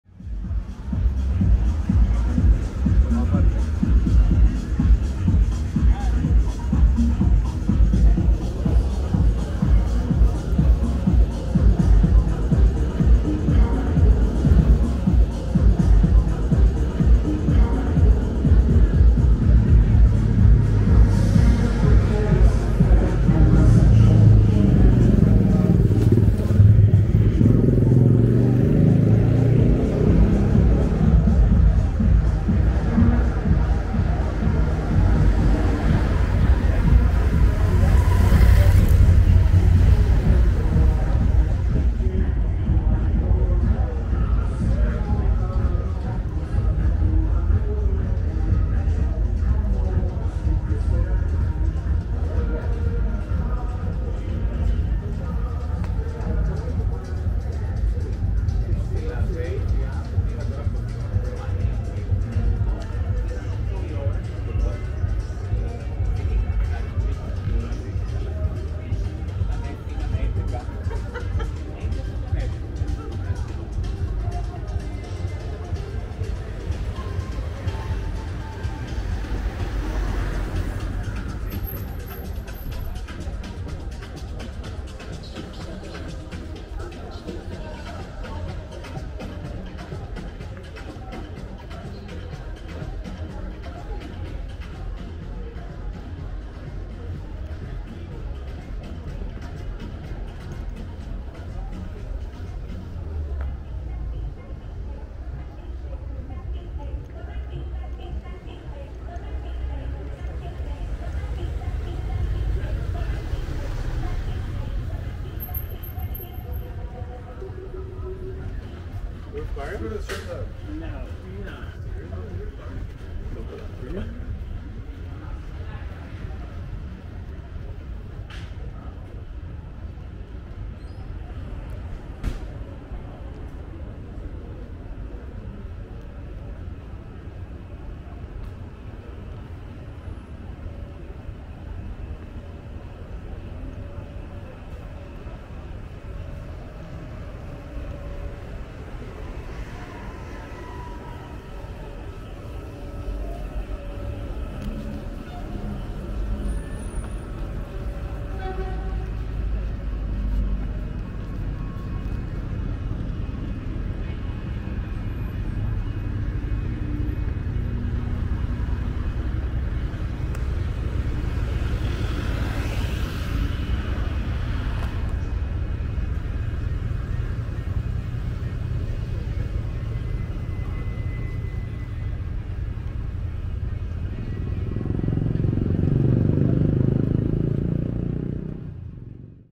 Walking in Greece. Walk around Kerameikos Metro Station at Night in Athens 1. ORANGE ua
Walk around Kerameikos Metro Station at Night in Athens 1